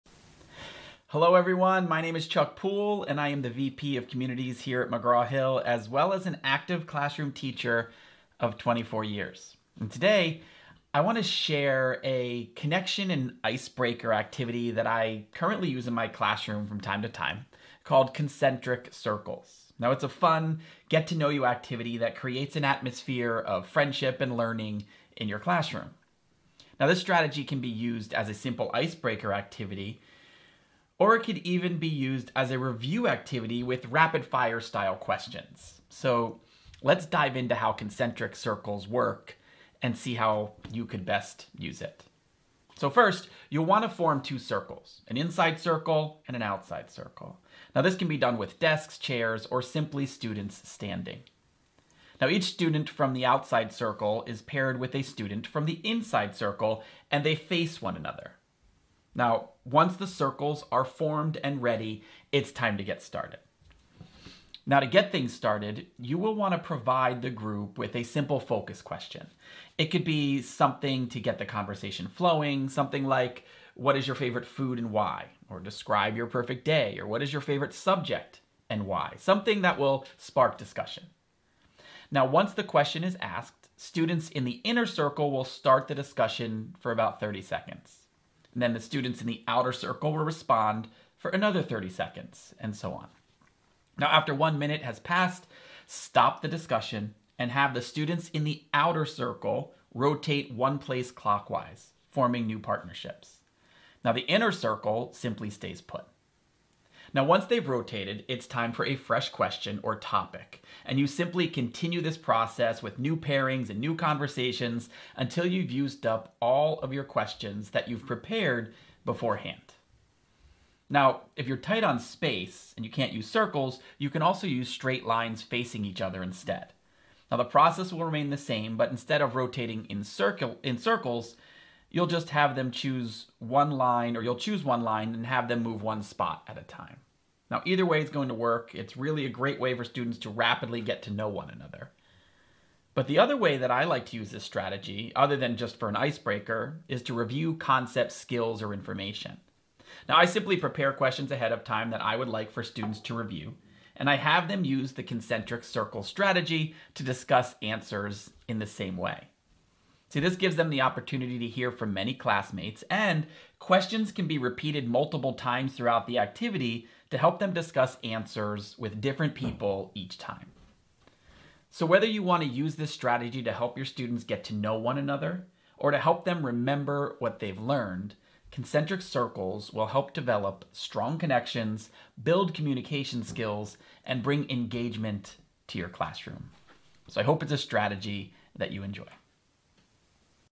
Teacher Toolkit: Concentric Circles
Practical, actionable teacher tips from Chuck Poole, VP of Communities at McGraw Hill.